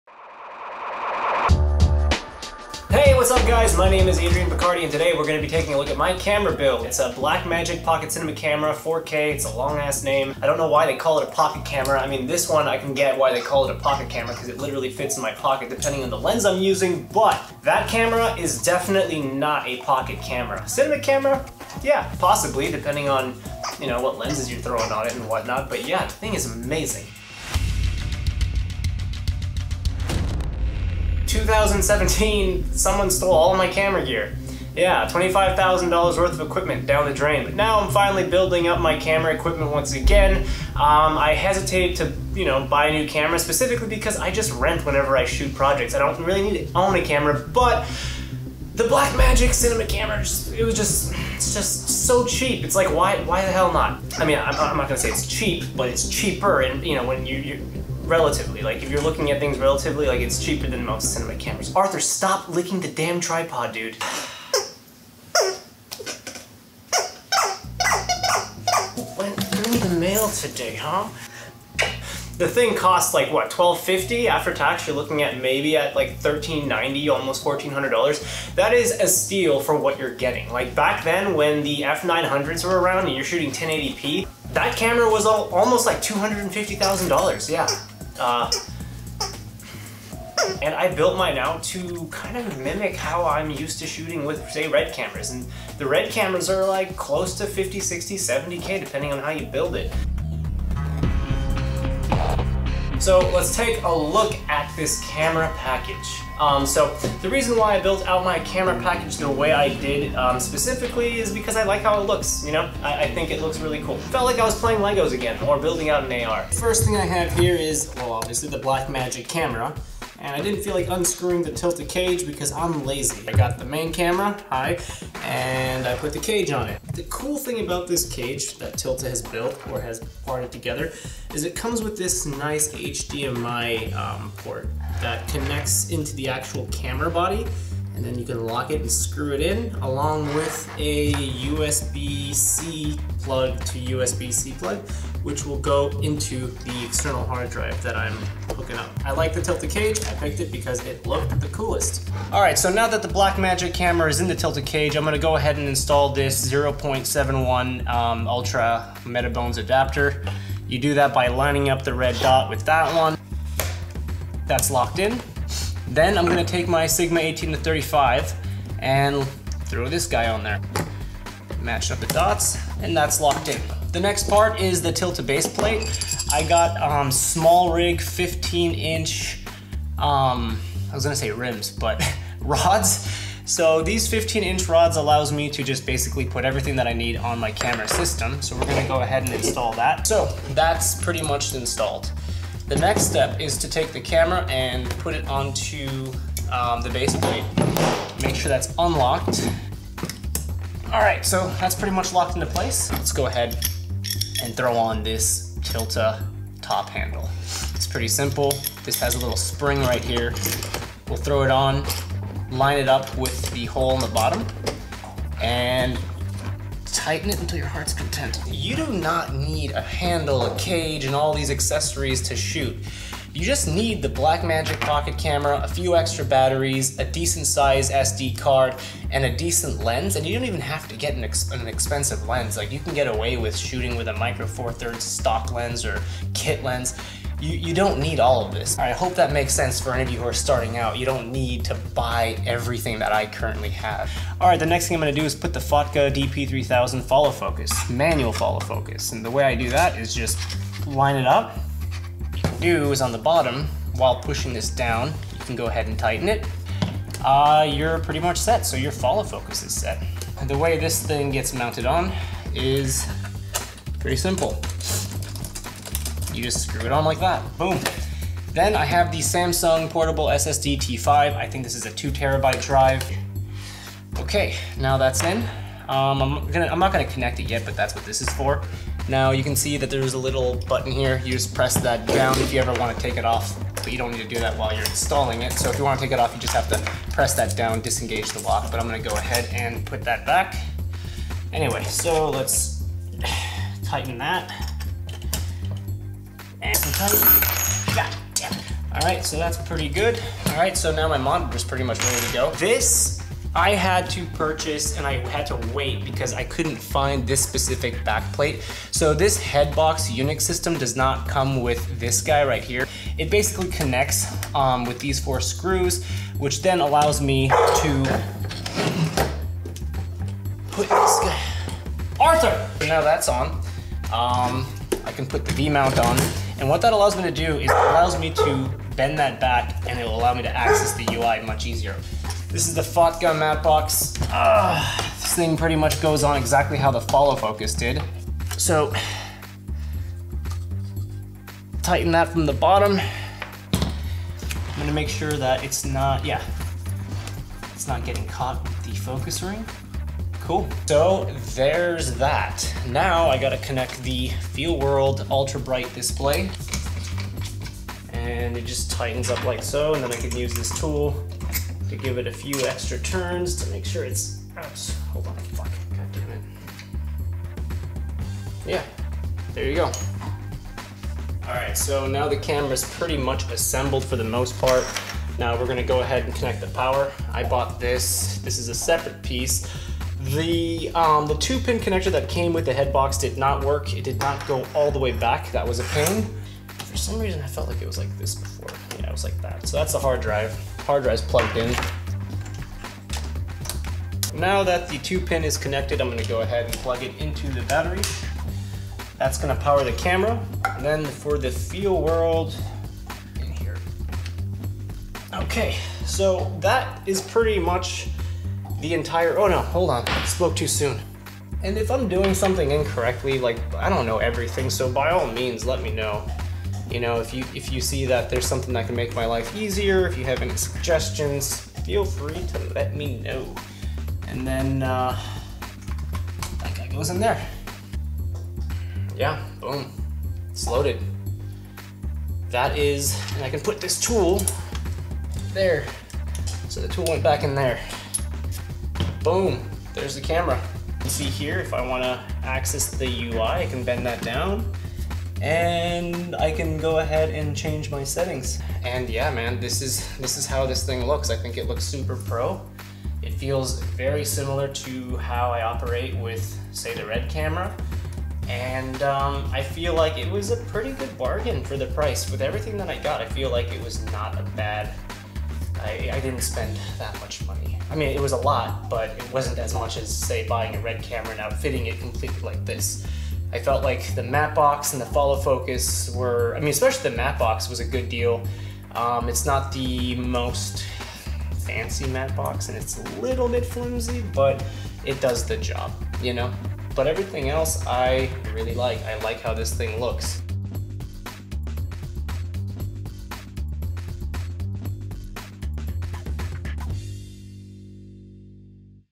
MY BUDGET CINEMA RIG - BMPCC4K
Finally built a new camera rig.
Let me know what you think or if you have any suggestions!
Links for all the parts is in the PDF below.